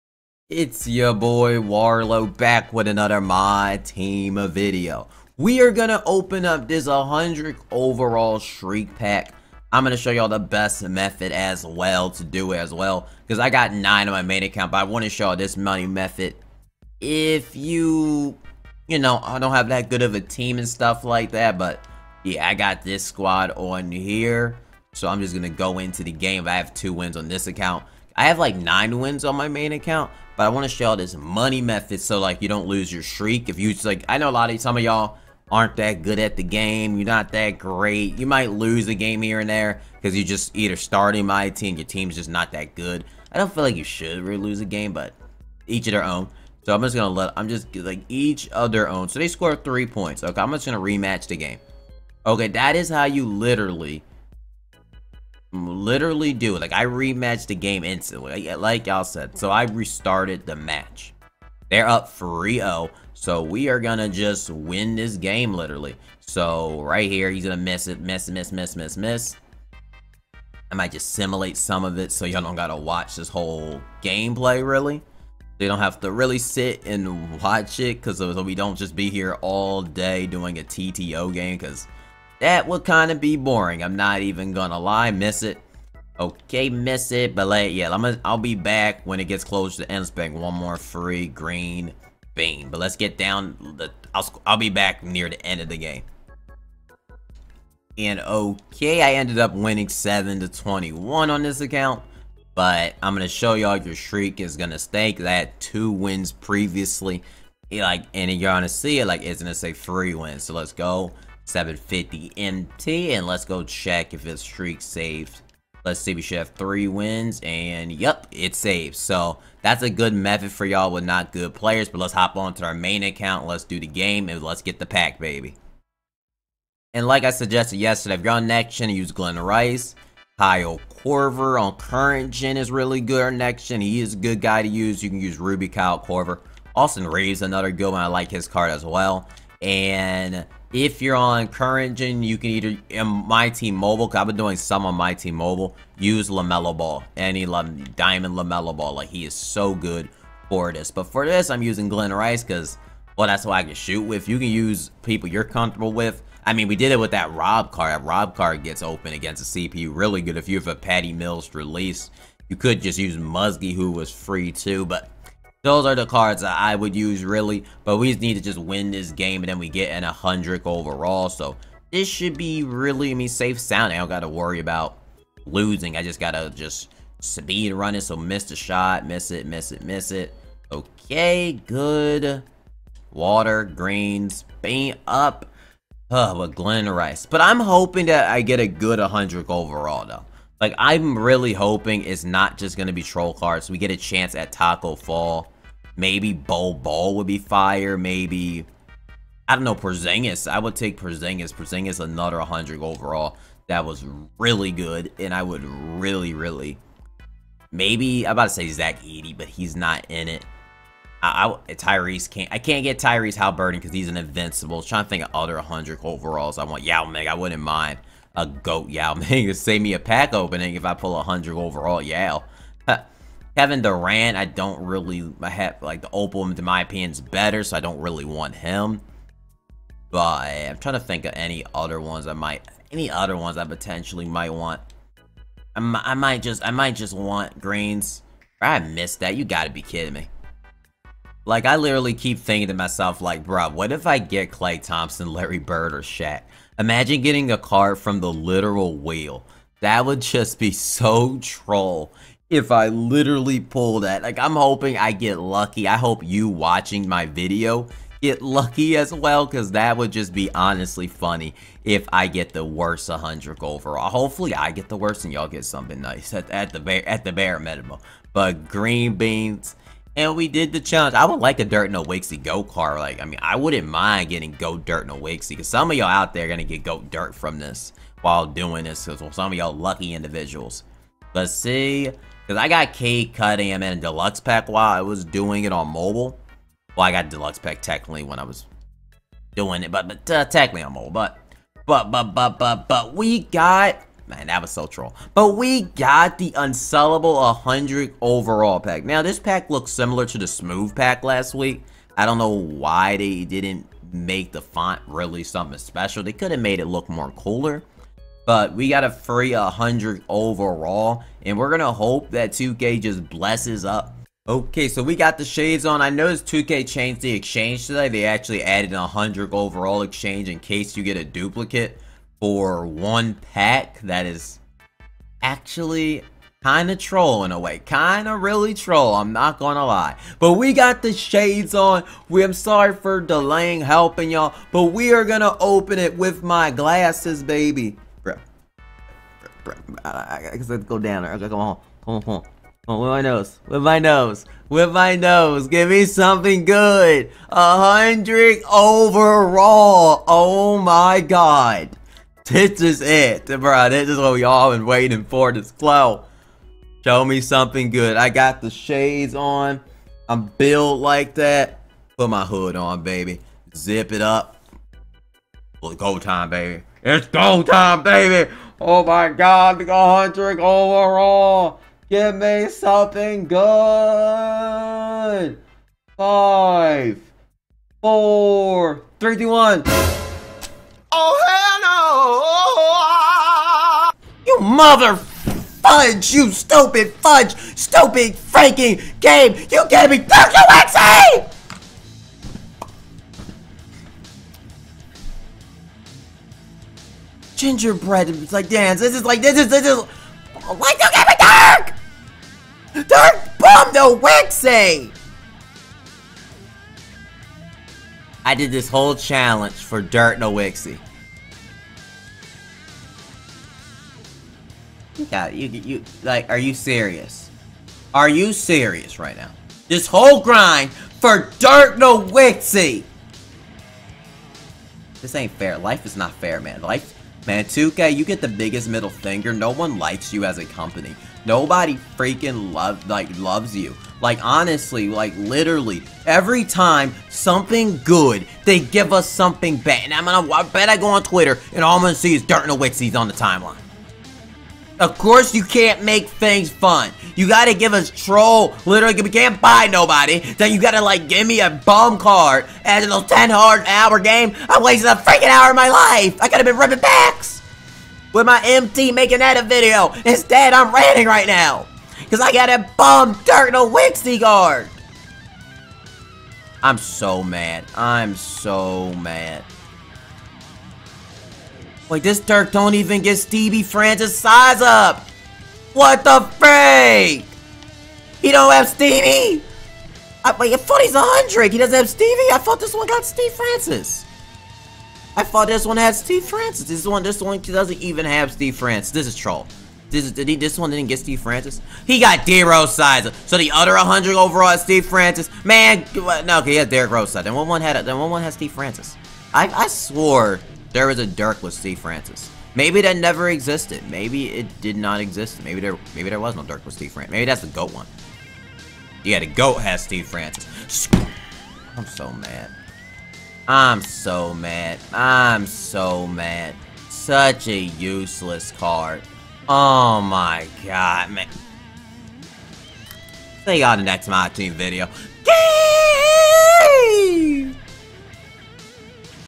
I OPEN THE *FREE* 100 OVERALL PACK! BEST METHOD TO NOT LOSE A GAME IN ONE HOUR
I GOT 10 WIN STREAK FOR 100 OVERALL BUT WAS IT WORTH IT. FIND OUT IN THIS VIDEO AND BEST METHOD TO DO IT